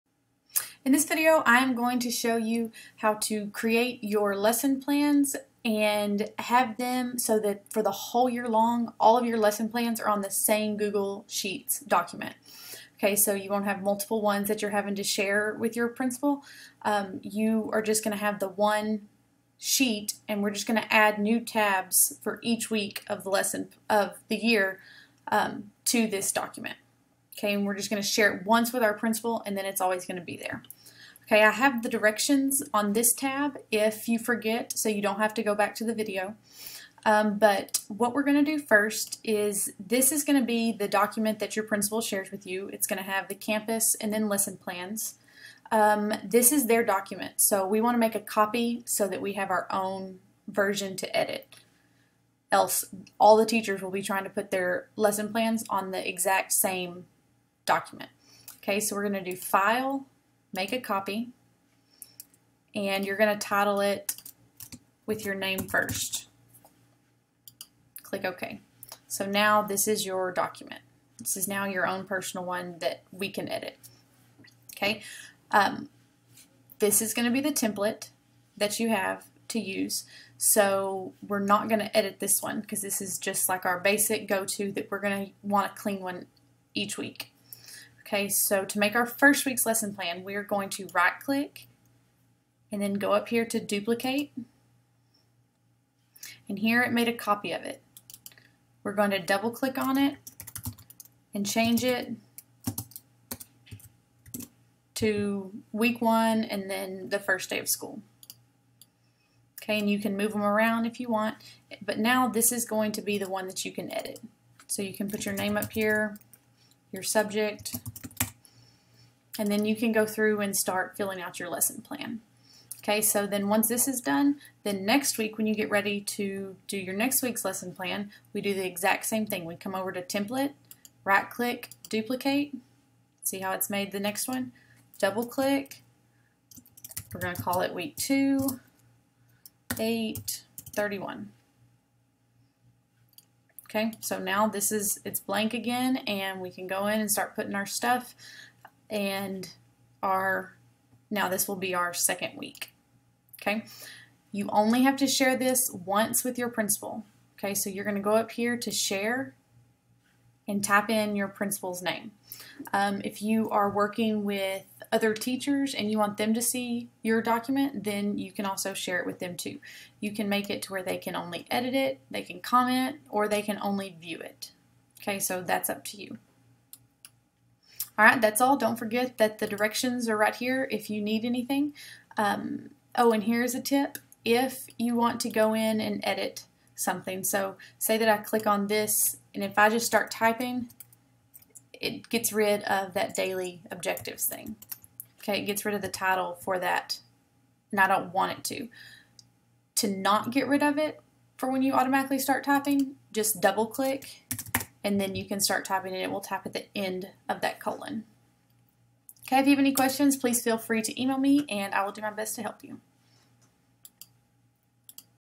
Creating Lesson Plans from a Template in Google Sheets
Create your weekly lesson plans using Google Sheets. In this video I show you how to use a template and make a new sheet for each week of the school year. You can share this document with your appraiser (only have to do it once, not each week!) and they can view your lesson plans.